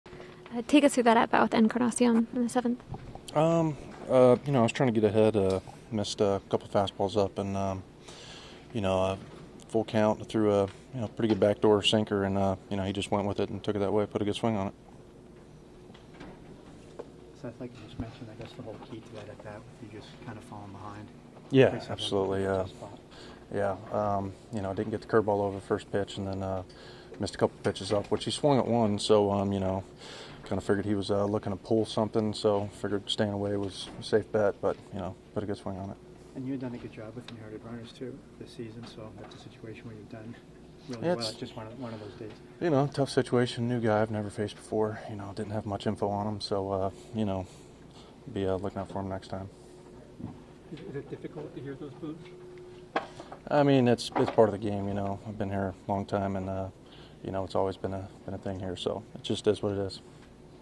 Seth Lugo explains pitch sequence that led to Jerar Encarnacion grand slam | Mets Post Game
In this Mets post game news conference, Seth Lugo tried to break down his strategy in pitching to Marlins rookie Jerar Encarnacion, before allowing an opposite-field grand slam that put Miami ahead for good. Lugo: "Full count, I threw a pretty good backdoor sinker, he just took it that way and put a good swing on it."